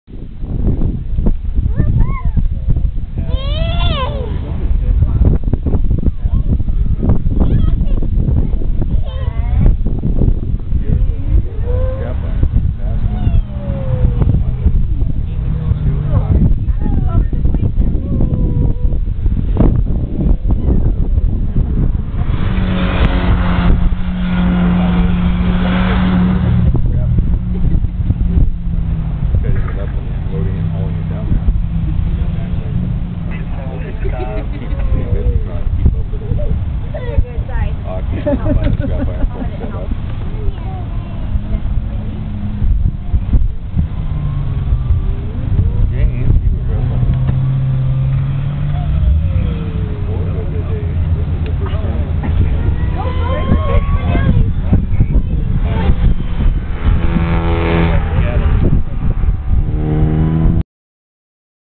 85 mr2 turbo
third run just started to spin out at the end when video stops.